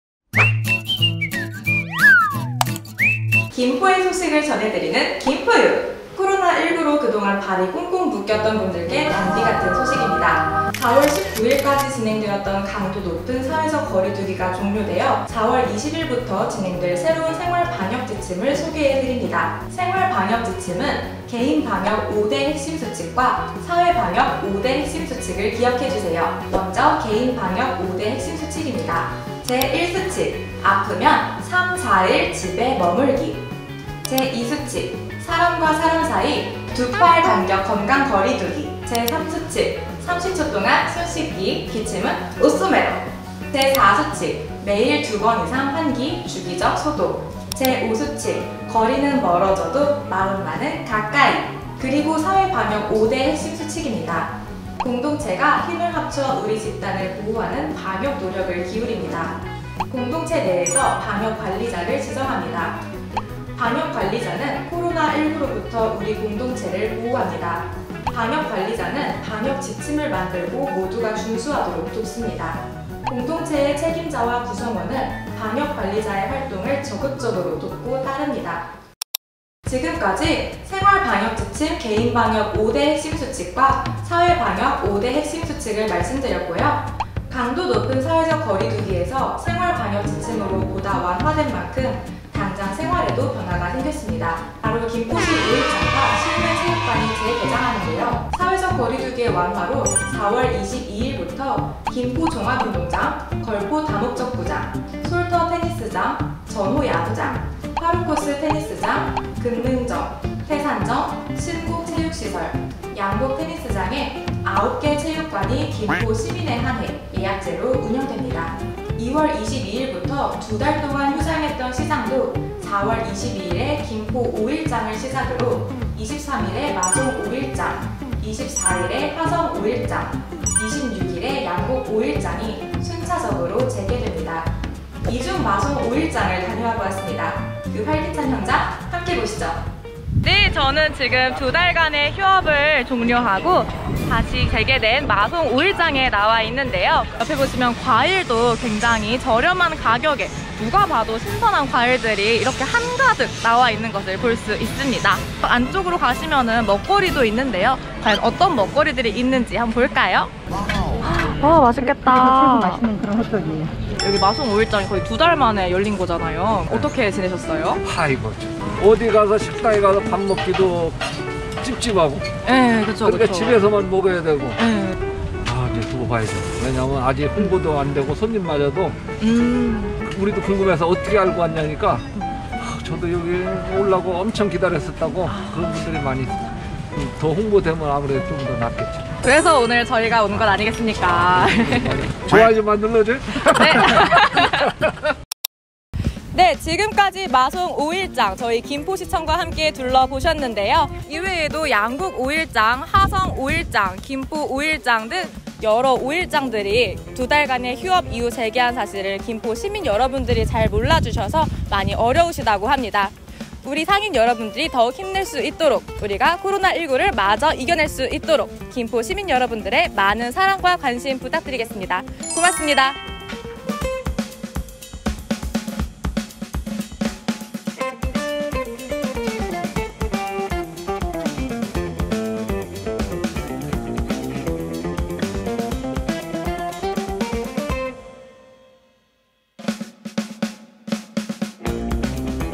[김포유] '두둥등장' 재개장한 마송5일장과 생활방역수칙!
4월 19일까지 진행되었던 강도높은 사회적 거리두기가 종료되고

4월 20일부터 진행되는 새로운 생활방역지침!

그와 함께 긴 휴장을 끝내고 재개장하는 김포의 시장들과 체육시설까지!

우리 모두 조금만 더 힘내서 코로나 이겨내자구요(빠샤)